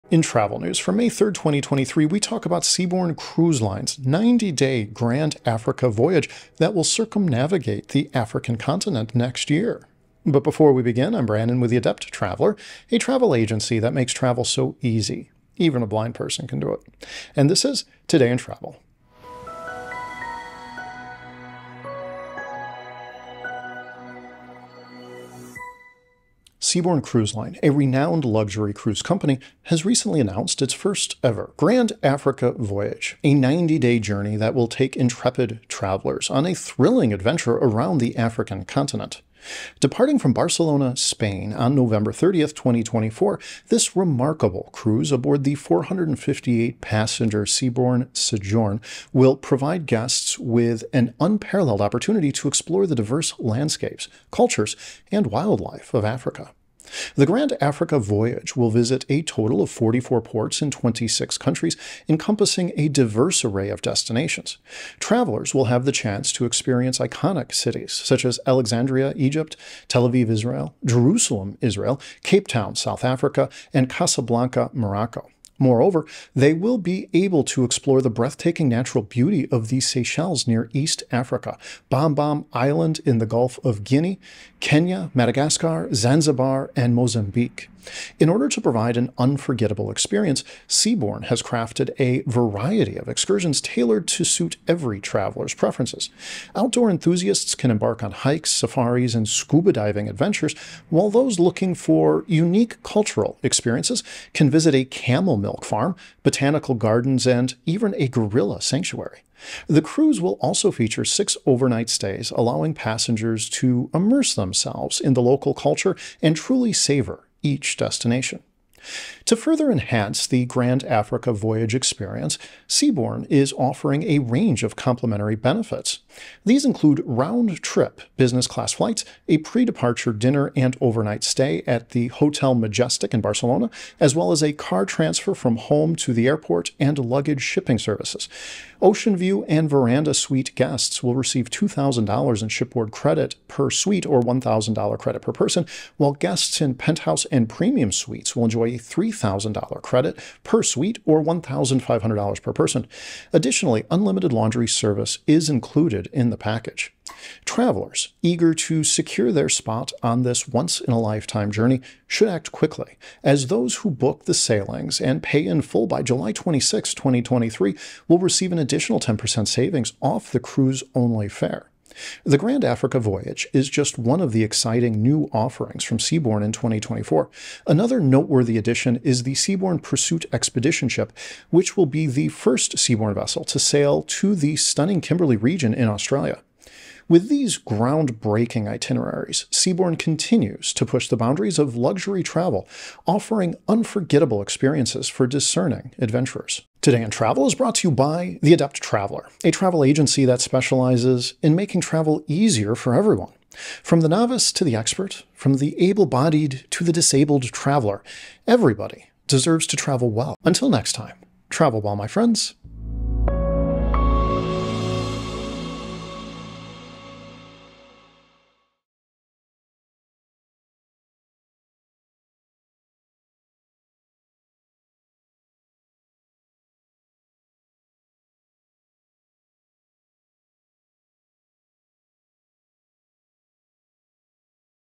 Embark on Seabourn's Epic 90-Day Grand Africa Voyage | Sailing from Barcelona in 2024!
Join us as we explore Seabourn's first-ever Grand Africa Voyage! This 90-day journey will take you to 44 ports in 26 countries aboard the luxurious Seabourn Sojourn. Discover vibrant cities, breathtaking landscapes, and experience unforgettable excursions. Get ready to embark on the adventure of a lifetime, departing from Barcelona on November 30, 2024. Book now to enjoy exclusive benefits and savings!